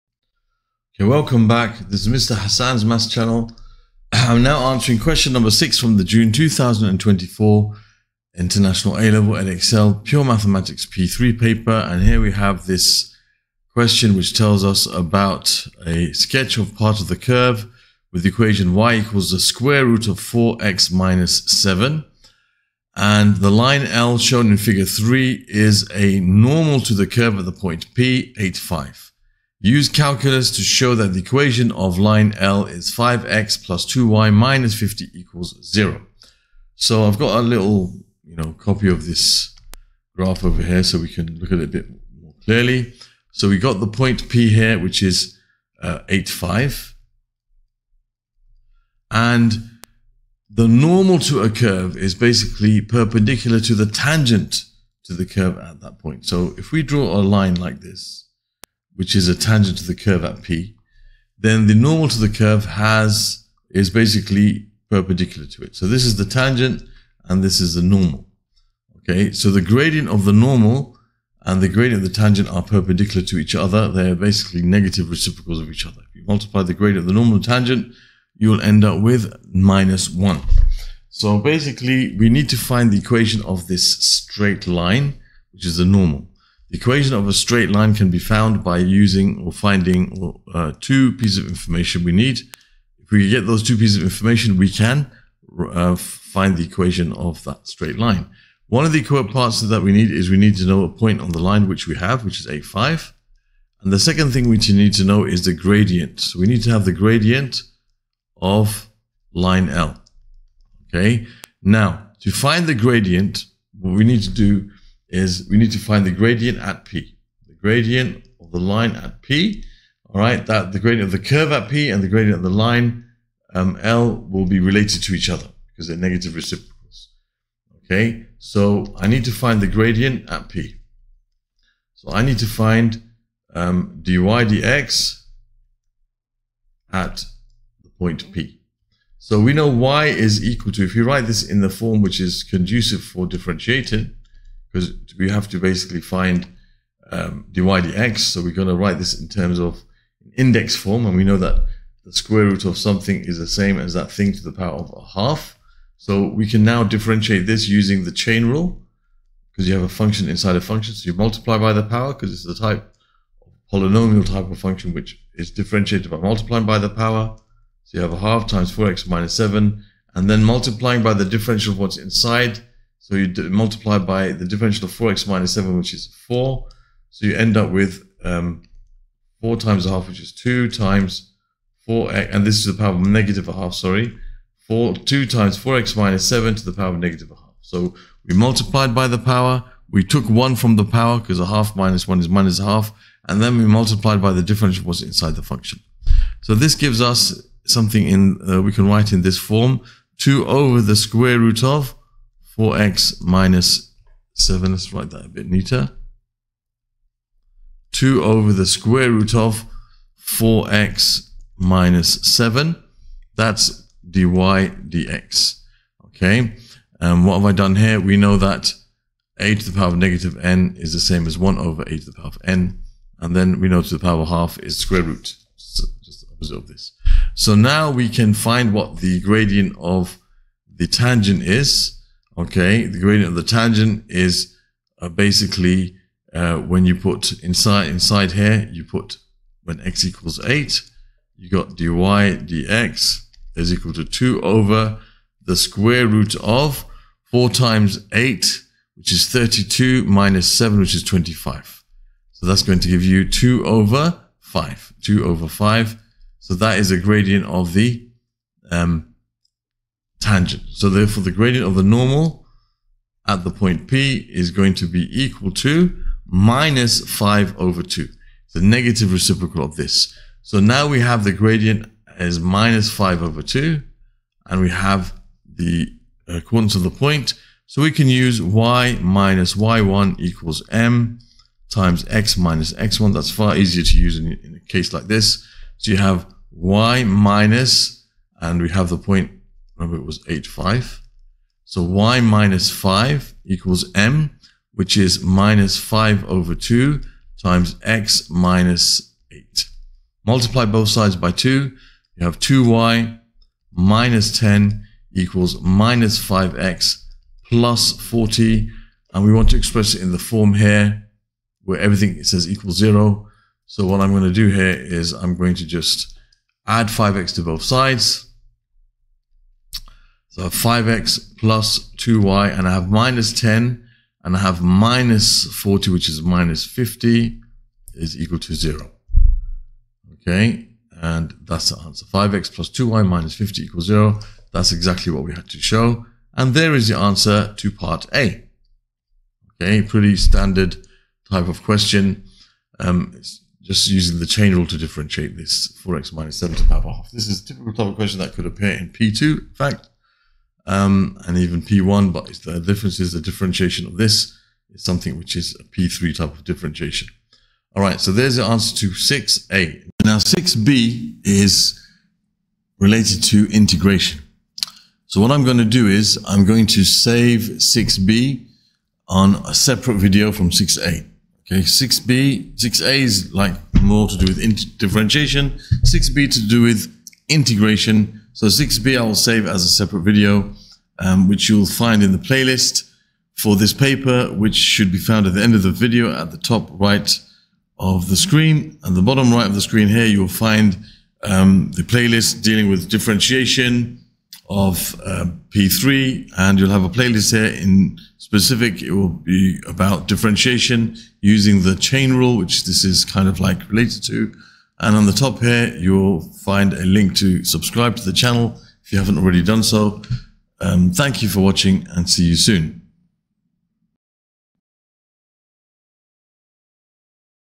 WMA13/01, (Edexcel), IAL, P3 June 2024, Q6(a), Differentiation, The Chain Rule, Equation of Normal
WMA13 01 IAL Edexcel P3 Check out the links at the end of the video to find playlists for questions on this same topic